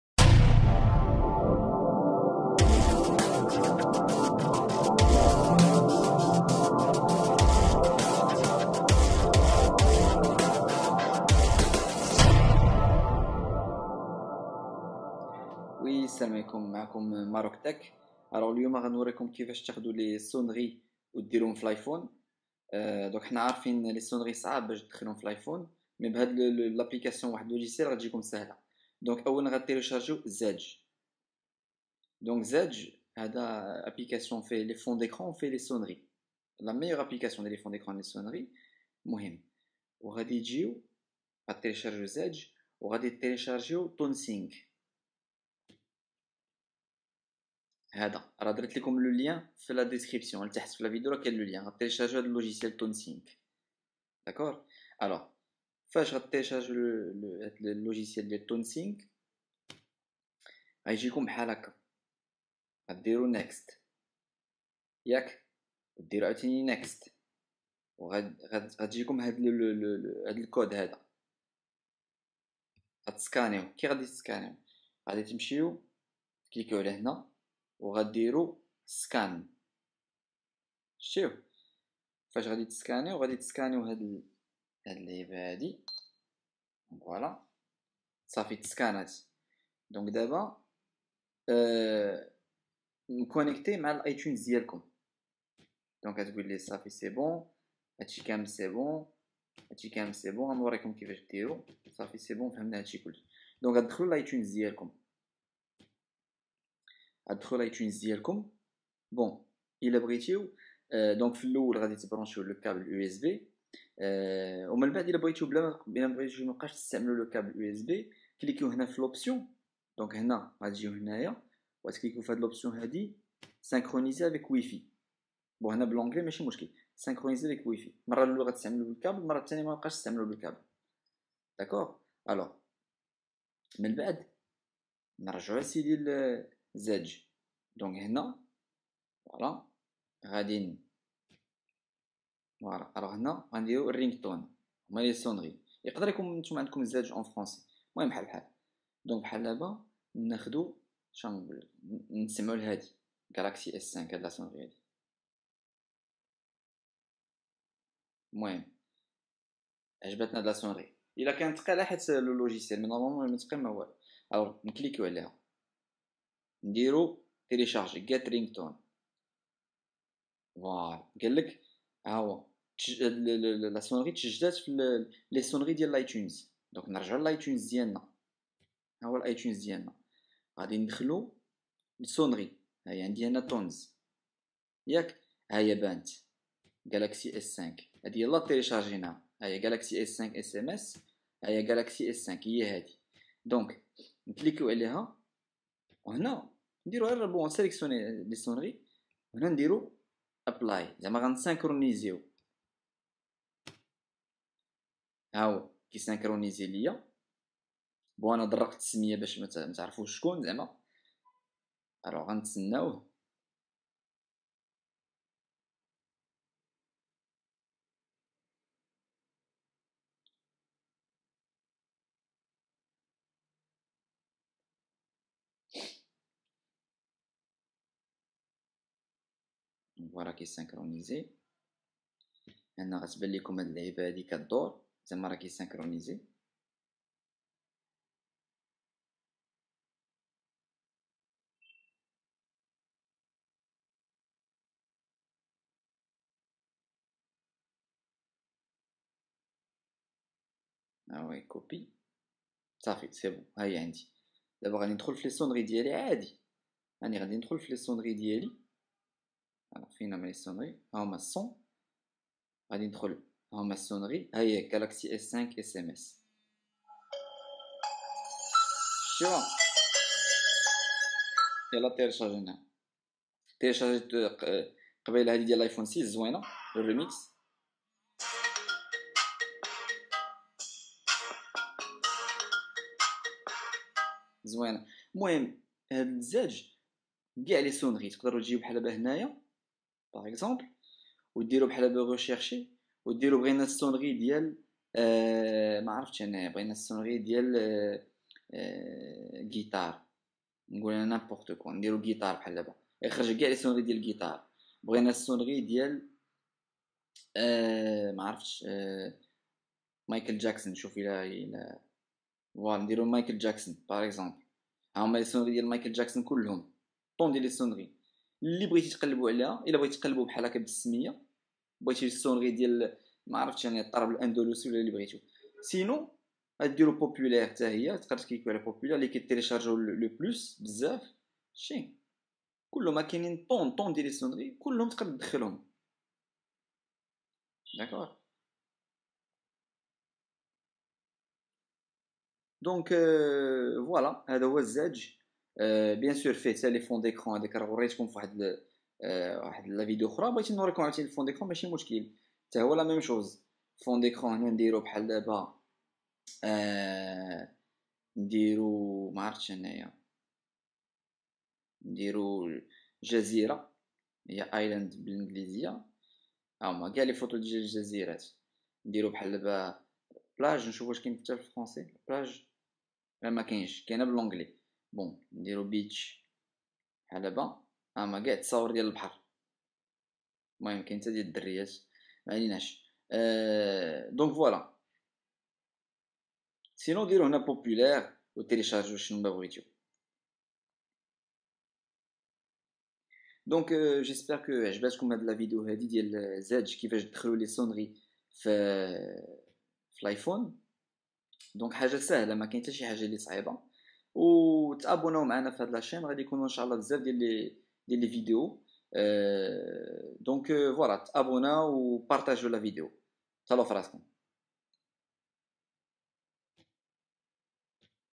Sonnerie Iphone Maroc (Arabe, Darija) ايفون المغرب
had la video fiha kifach dekhel les sonneries Iphone, sahla, tbeb3ou m3aya la video.